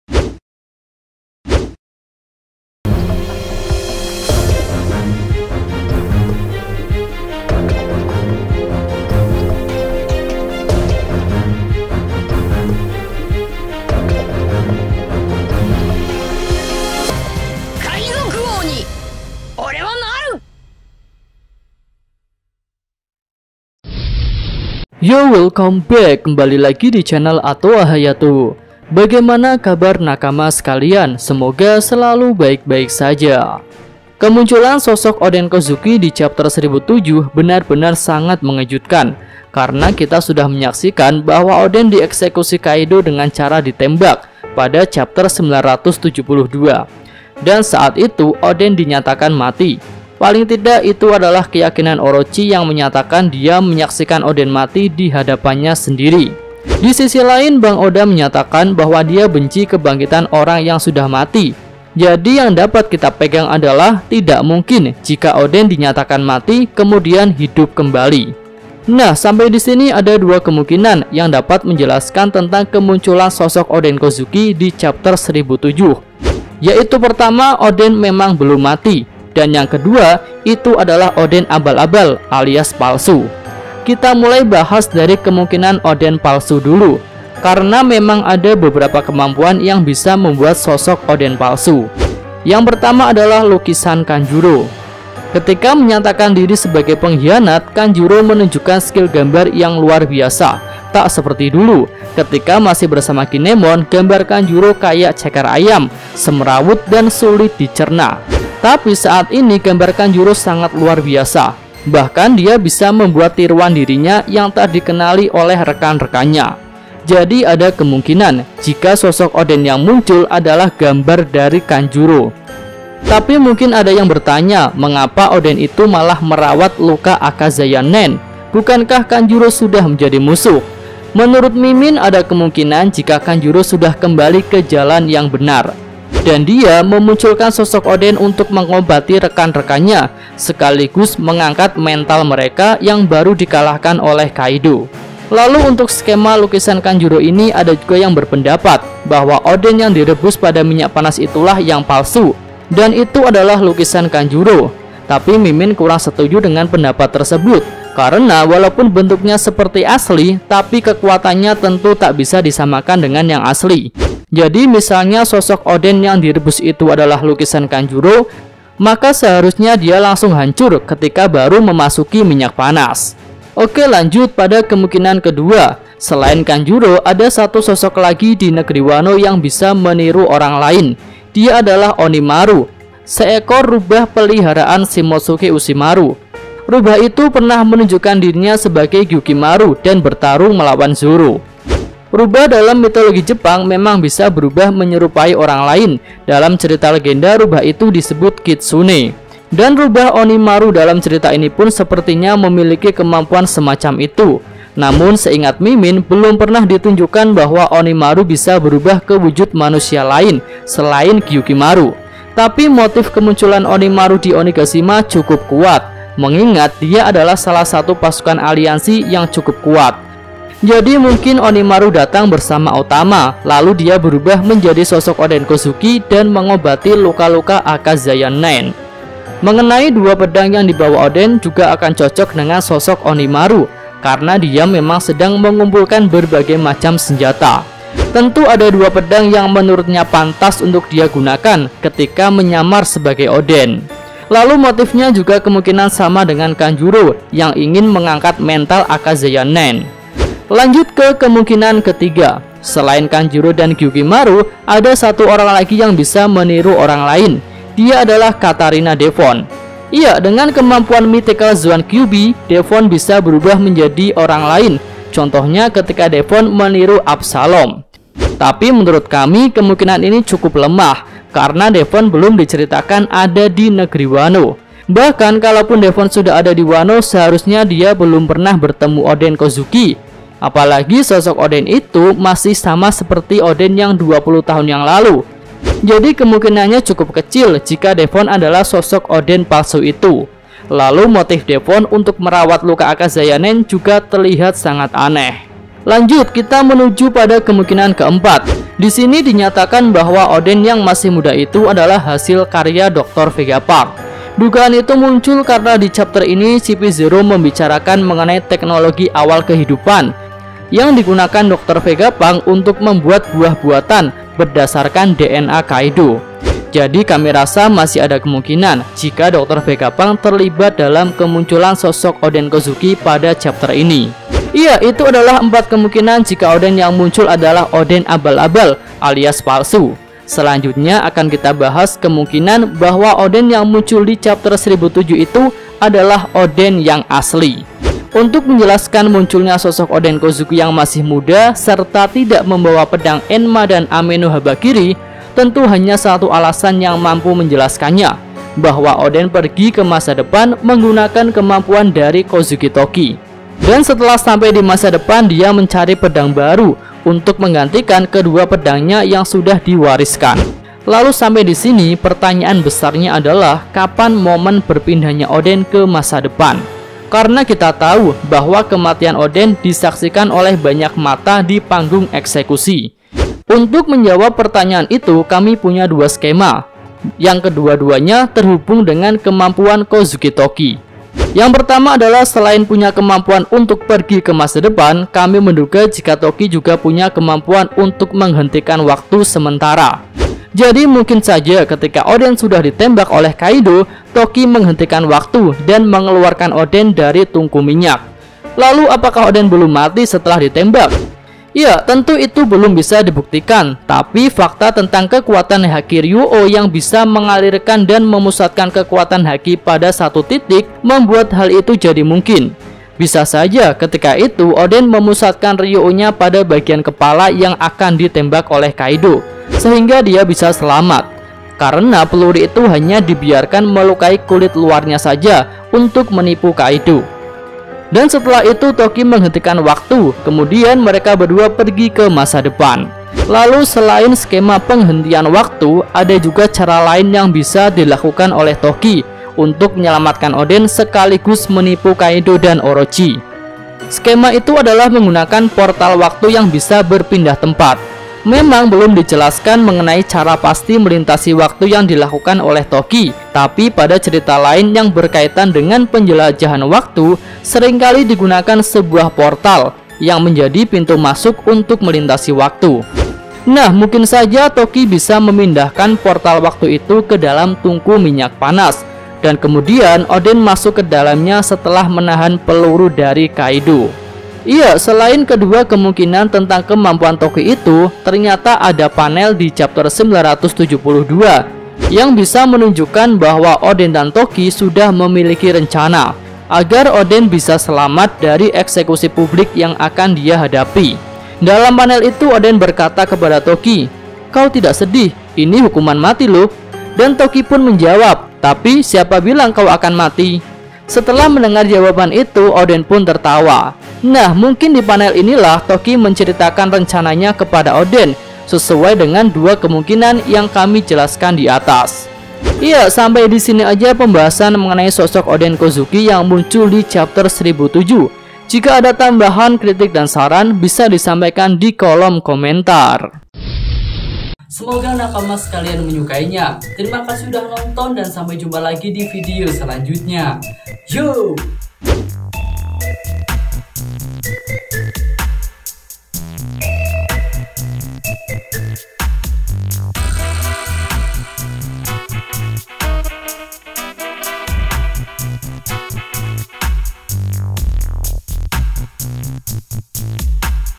LUAR BIASA!! INILAH CARA TOKI SELAMATKAN ODEN KOZUKI !?
kemunculan sosok oden di chapter 1007 sangat mengejutkan, dan kira-kira itu oden asli atau palsu ya,.

simak ulasannya dalam video ini ,..